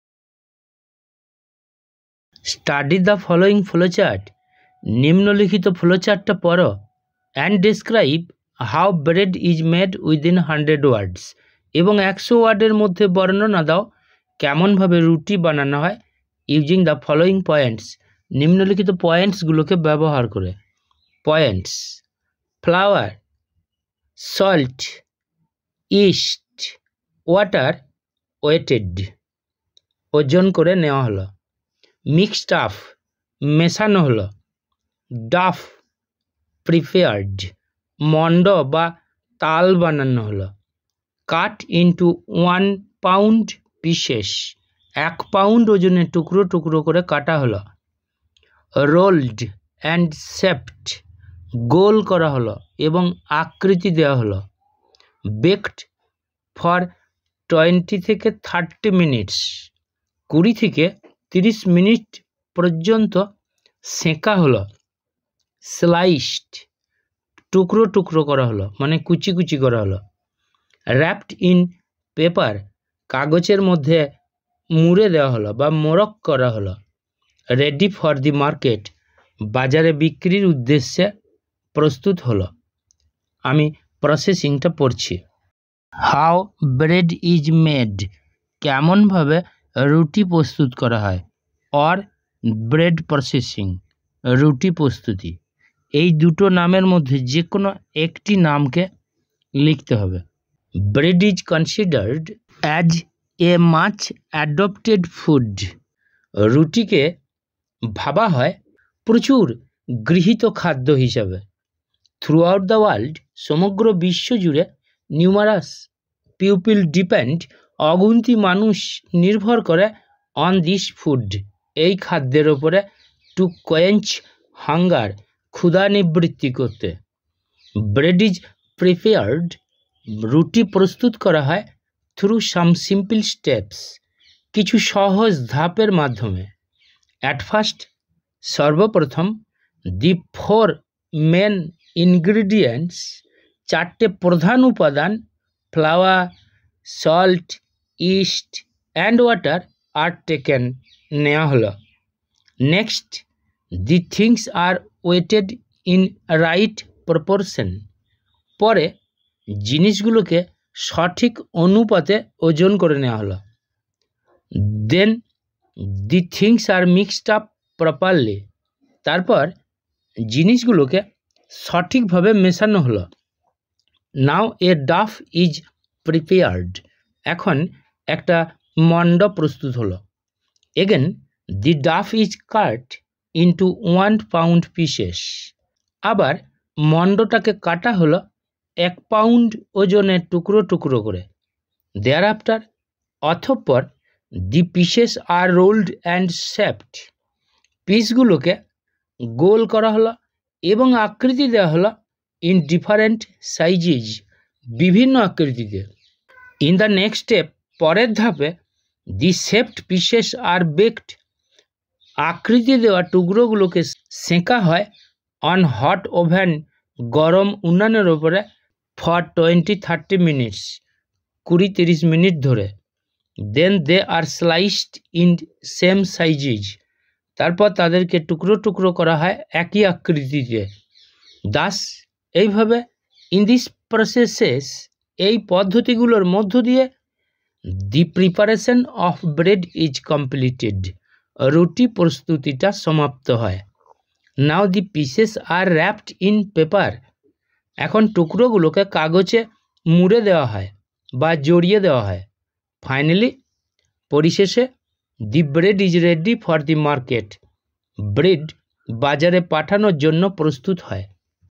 Describe / How Bread is made / Bread Processing preparation Writing / ABTA 2023 24 Page 154 - 157
9. Study the following flowchart and describe 'How Bread is made' (within 100 words) using the following points:

[Points: Flour (ময়দা), salt, yeast ইস্ট (ছত্রাক), water weighed -- mixed up -- dough (মন্ড/তাল) prepared -- cut into one pound pieces -- rolled and shaped -- baked for 20-30 minutes -- sliced -- wrapped in paper -- ready for the market]

Madhyamik ABTA Test Paper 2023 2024
Paragraph Writing